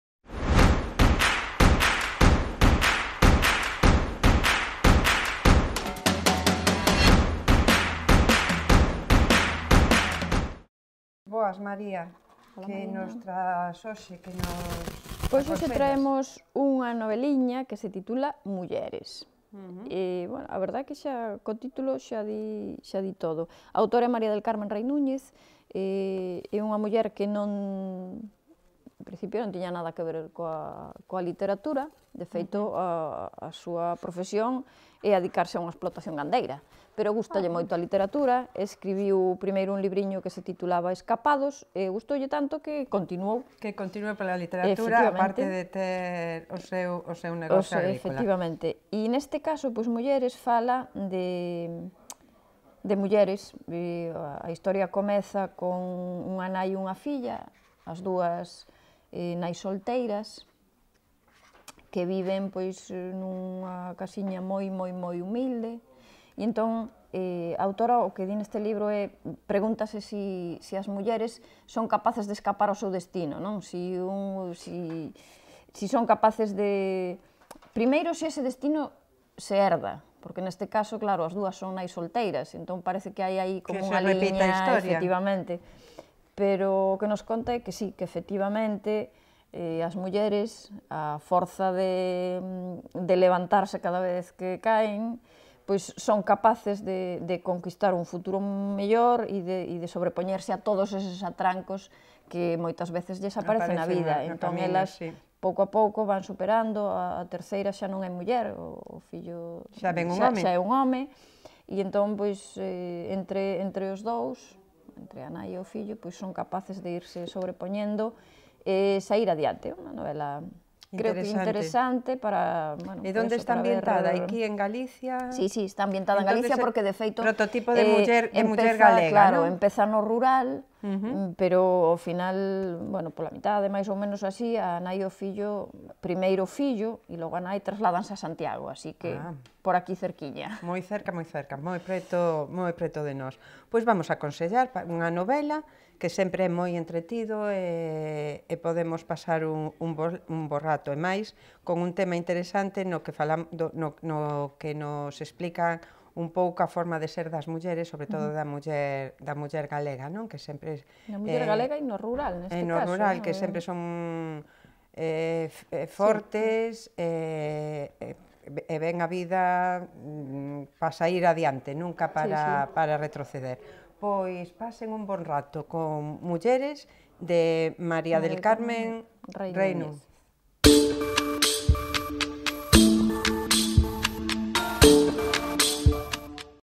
Libraría Couceiro: Mulleres
'Mulleres', unha novela da autoria de María del Carmen Rey Núñez, é a proposta da libraría Couceiro, para ler unha historia de mulleres arriscadas, atrevidas e valentes que collen as rendas da súa vida a pesar das dificultades.